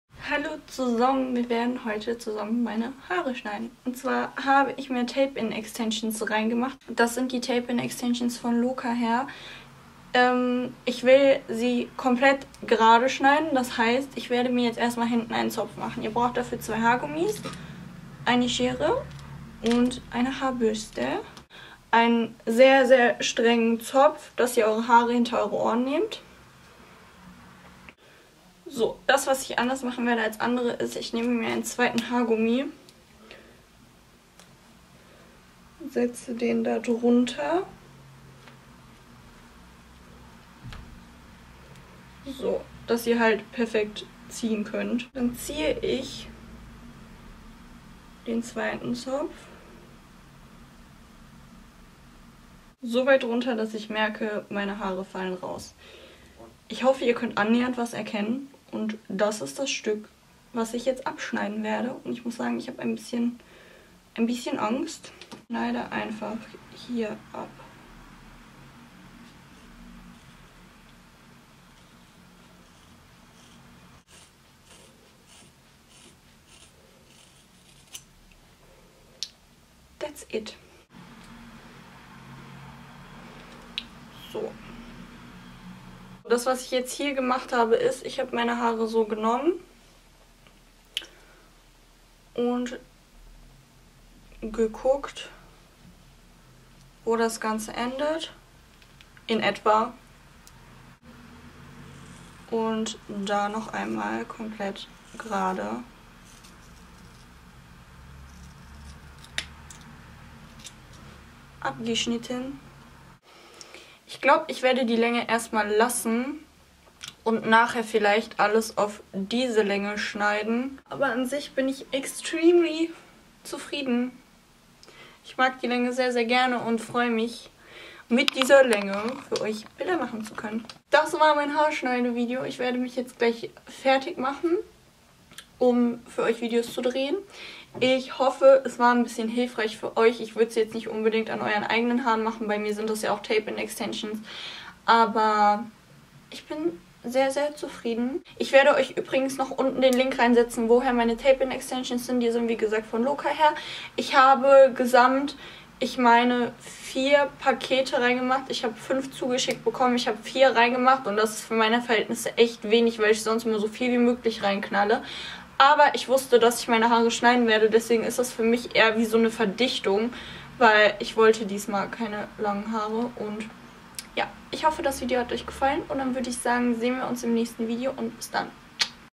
Ich schneide meine Haare 😳✂️💇🏻‍♀️
Heute nehme ich euch mit, wie ich meine Haare schneide. Und freue mich wenn ihr mir auf meinen anderen Social Media Accounts folgt, als auch auf YouTube. :-)

Hier findet ihr meine anderen Profile ♥

xoxo
Nessa